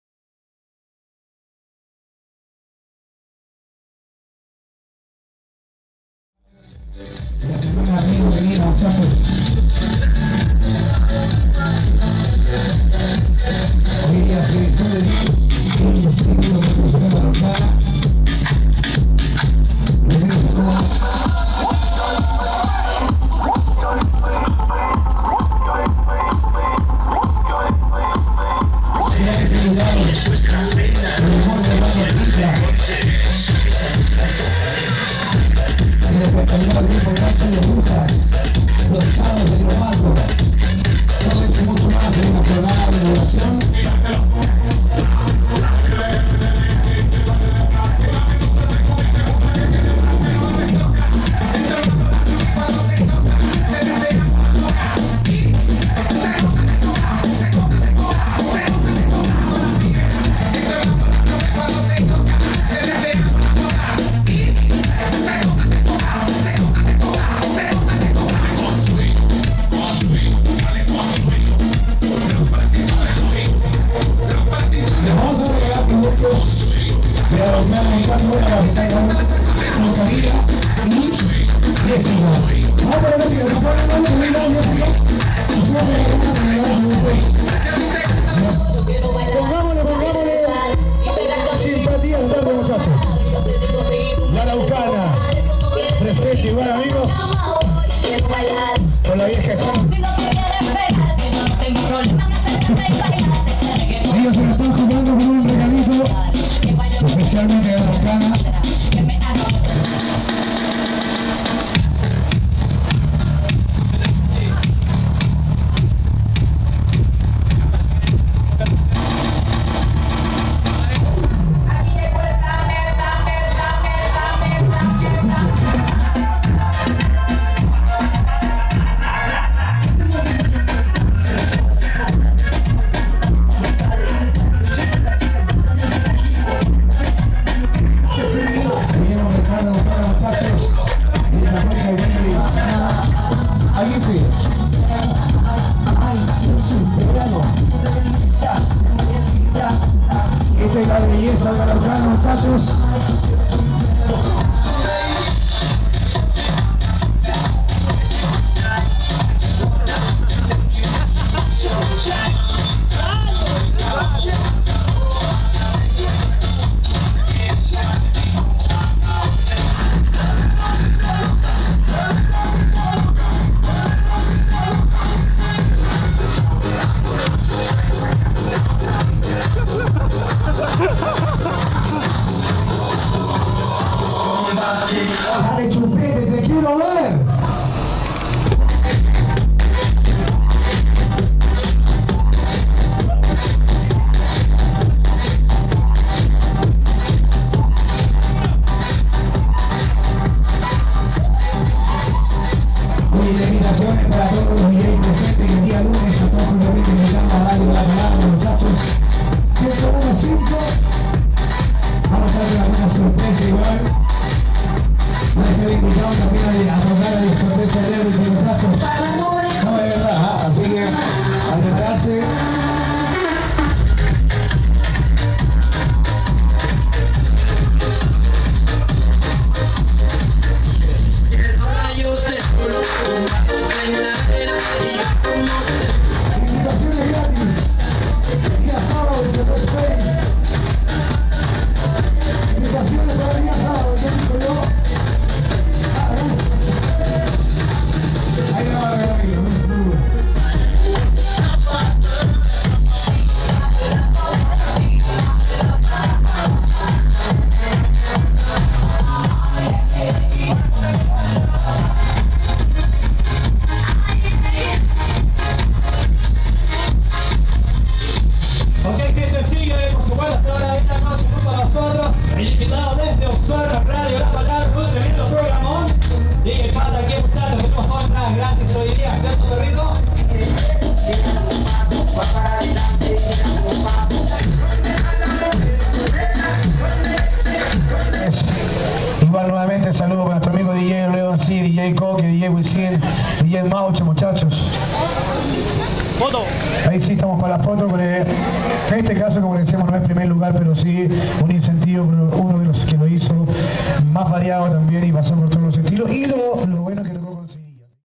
ENCUENTRO DE DJs RIO NEGRO 2012.m2t
Encuentro de Djs se denomino la actividad, que  con publico masivo se realizo en la plaza de armas de la comuna de Río Negro, actividad que busco revivir los antiguos encuentros realizados hace años atrás, en la ocasión participaron en la mayoría djs locales y de Osorno la participación de Dj Pata, el evento fue organizado por la municipalidad de rio negro y contó con el auspicio de Space Discoteque y La Araucana